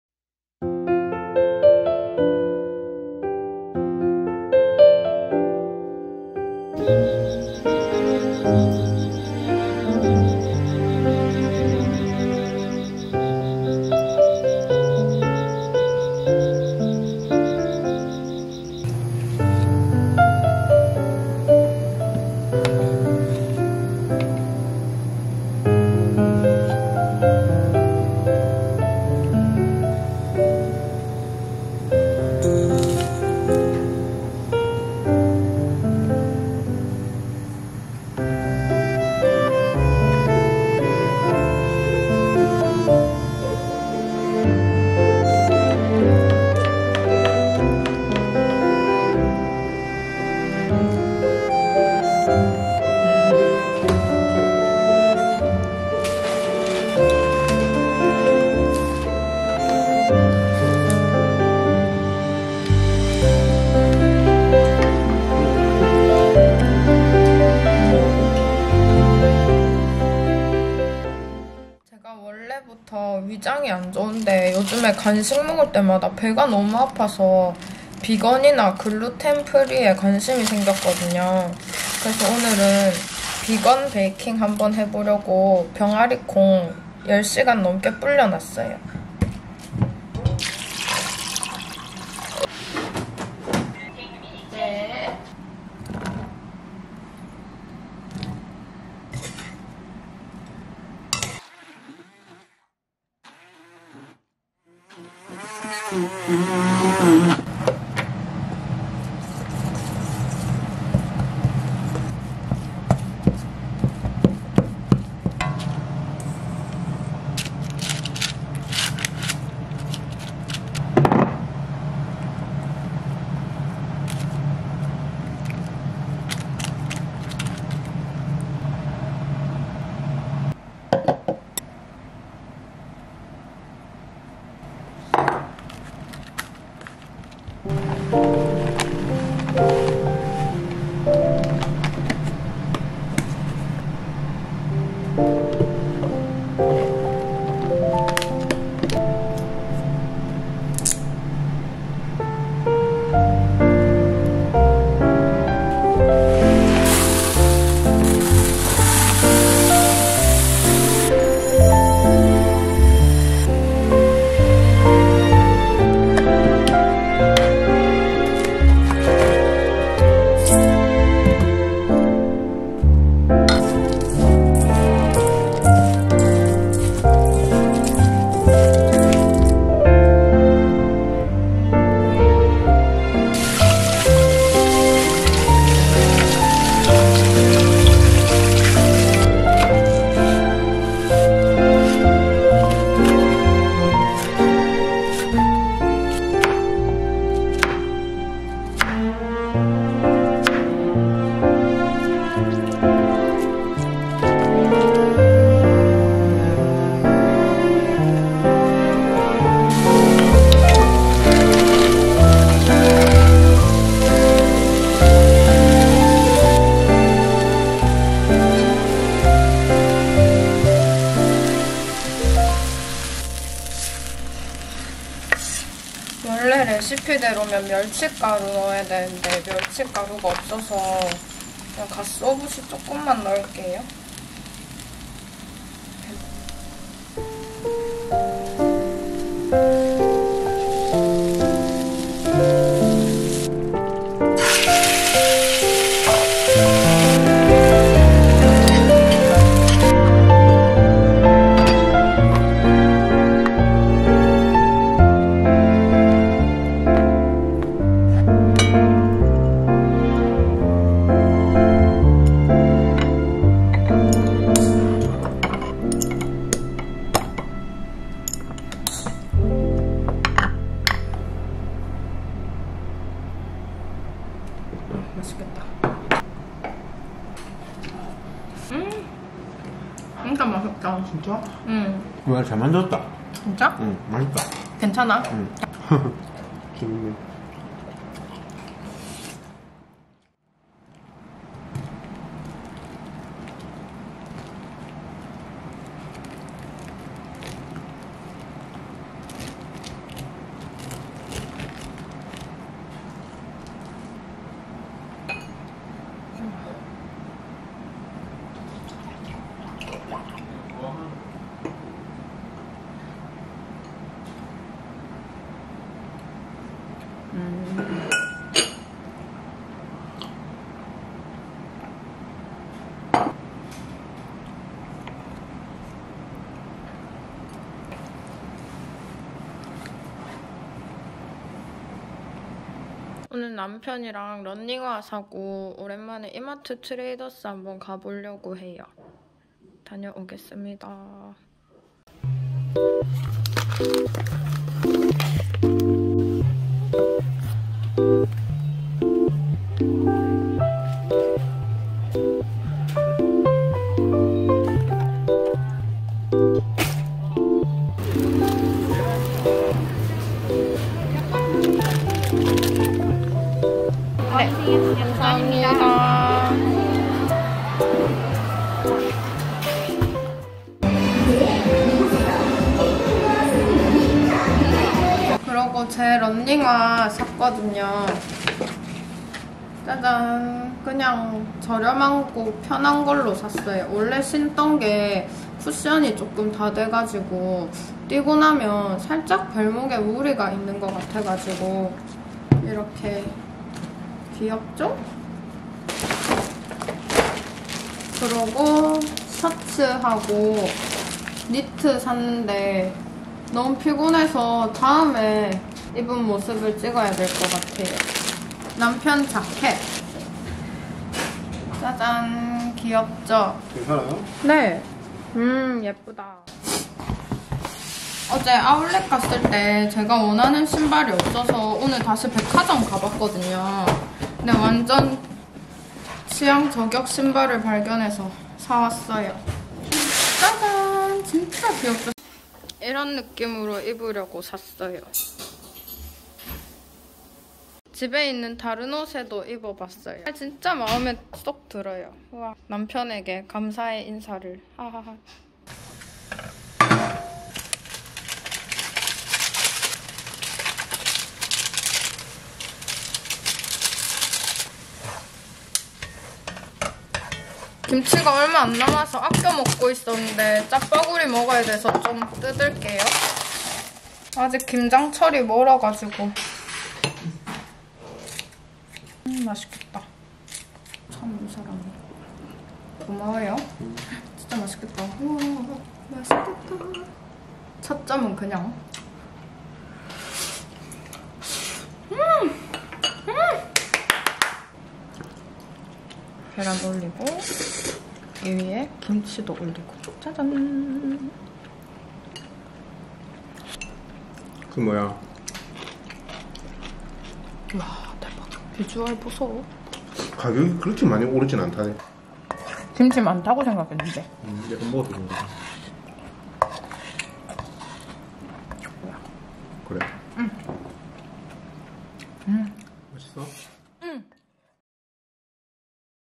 신혼부부 브이로그|첫 비건베이킹, 우렁쌈장, 짜파구리, 가을 옷 쇼핑하는 일상
안녕하세요.
한주간 잘 지내셨나요?

이번주는 우렁쌈장 만들어봤는데 너무 맛있었어요!

그리고 요즘 다시 위장이 안좋아져서
비건베이킹에 도전해봤답니다ㅎㅎ

속도 편안하고 맛있었어요!

가을 옷도 조금 쇼핑하고 즐거운 주말을 보냈어요

영상 재밌게 봐주세요!
***매주 목요일 7시에 업로드합니다.
인스타그램으로 소통해요!​